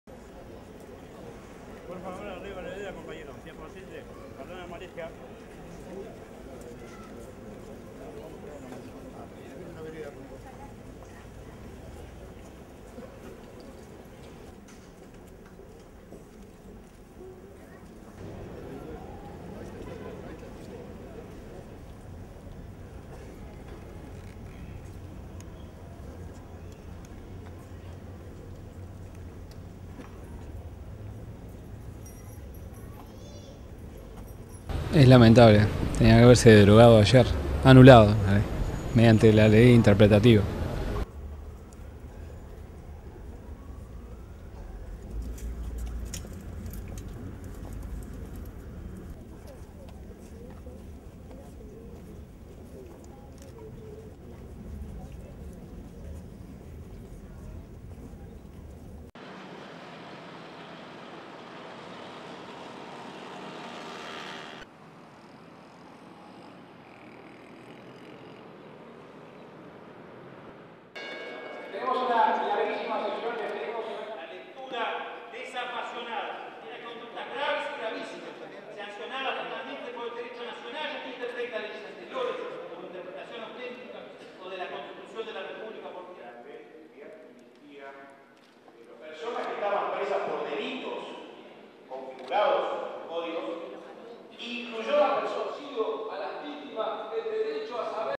Demo after bid to overturn amnesty for officials of frm dictatorship failed
++NIGHT SHOTS++
1. Zoom out of silent march with demonstrators holding photographs of people who disappeared during dictatorship led by banner that reads (Spanish) "Truth and Justice. The right of everybody".
2. Various of march and people holding up photos of the missing
3. Wide of street scenes
4. Various of demonstrators marching down the street
5. SOUNDBITE: (Spanish) Vox Pop, demonstrator:
"It is regrettable. It should have been annulled yesterday."
6. Various of march
7. Various of demonstrators with photos of people's eyes
++INTERIOR SHOTS++
May, 20, 2011
8. Various exteriors of congress building
9. Various of interiors of congress during debate on amnesty laws.
STORYLINE
Thousands of demonstrators marched on Friday in Montevideo, Uruguay, a day after a proposal to annul an amnesty for officials of the country's former dictatorship fell one vote short in Congress.
The vote came after a bitter debate that has reopened divisions from the 1973-85 military government.
In 2003 a peace commission found that the dictatorship killed 175 leftist political activists, 26 of them in clandestine torture centres.
About 150 Uruguayans met the same fate at the hands of a junta in neighbouring Argentina.
In 2009 the Supreme Court ruled that the amnesty in place for dictatorship officials was unconstitutional.
IN addition, the Inter-American Court of Human Rights ruled this year that Uruguay must remove roadblocks to prosecuting crimes against humanity.
Yet during Thursday's vote, the country's ruling centre-left Broad Front party ended up one vote short of a majority in the 99-seat legislature.
Demonstrators outside marched silently, with photographs held high of people who disappeared during Uruguay's military dictatorship.
Human rights groups argue the official amnesty in place allows government crimes to go unpunished.
But veteran groups and politicians, including President Jose Mujica, are concerned the measure of annulling the amnesty will create further political dangers.
In April, Uruguay's Senate approved the measure to annul the amnesty by a single vote.
Two popular plebiscites to overturn the amnesty failed to win majorities, but by thin margins.
Forty-six percent voted to overturn the amnesty in 1989 and 48 percent voted for the measure in 2009.
Friday's protests also marked the anniversary of the 1976 murder of two Uruguayan congressmen who were killed while in exile in Argentina, after a military government seized power there and cooperated with the dictatorship in charge of Uruguay at the time.